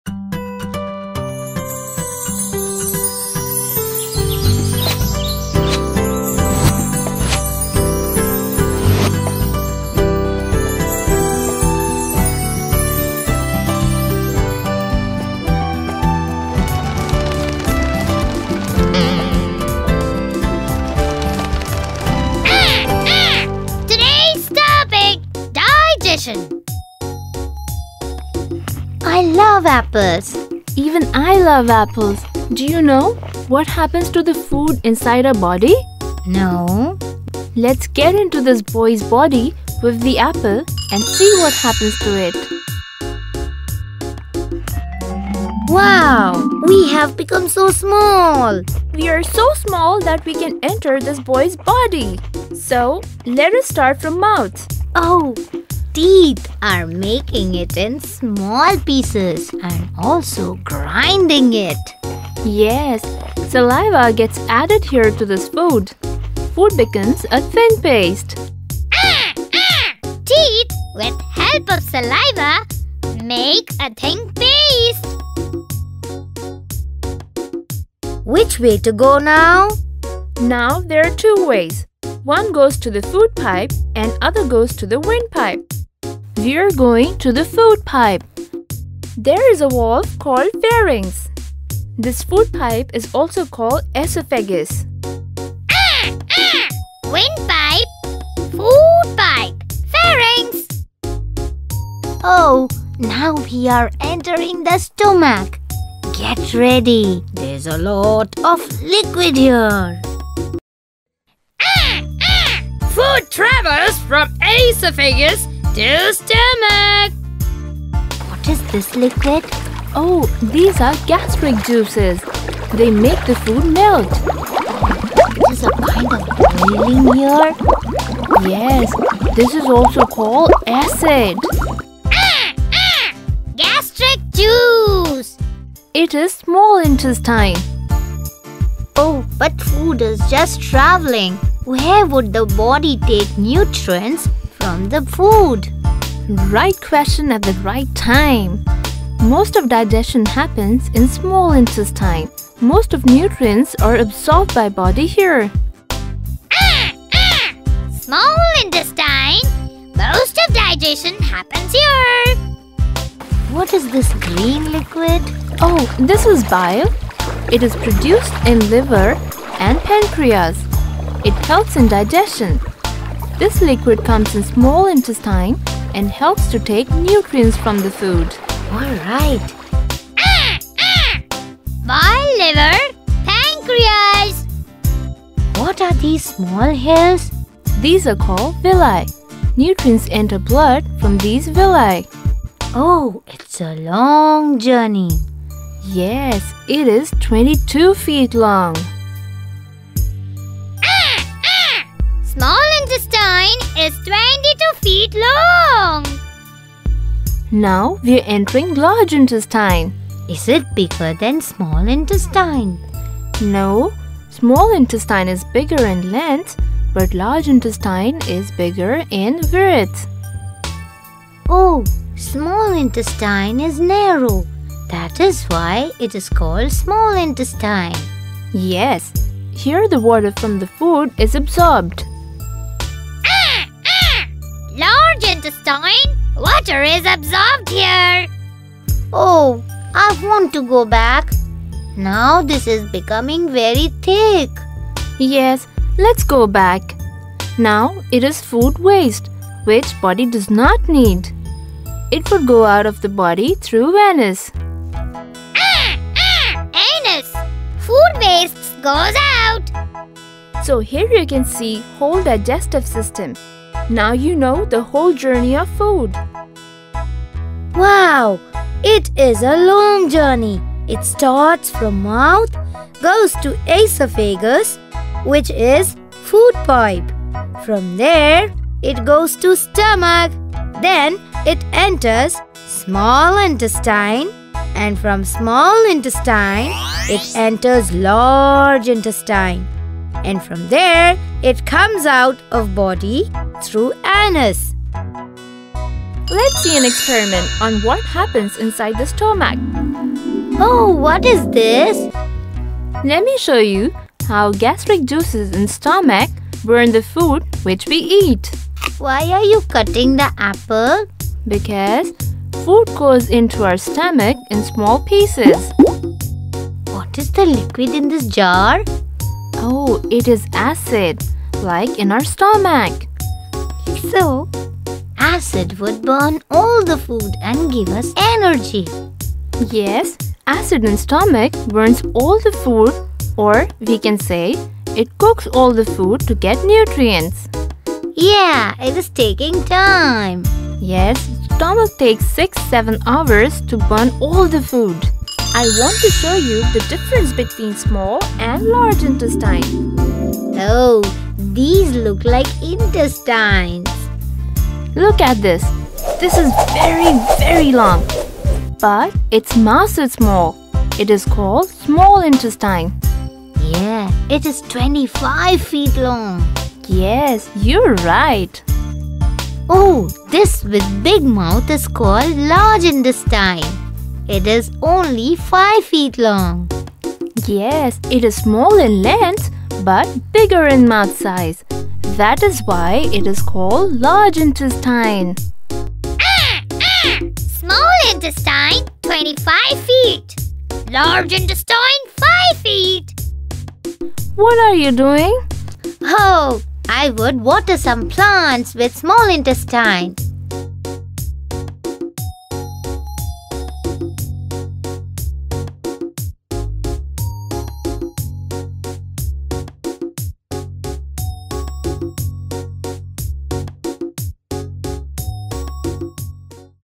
Digestive System -Human Body for Kids-How Body Works- makemegenius.com
science education websites.

Human digestive system starts working even before you take a bite of that  yummy pizza and it keeps working even after you've finished eating. The digestion process helps your body to get the nutrients and energy it needs from the food you eat.In this educational video,kids can go through the whole digestion order in an interactive way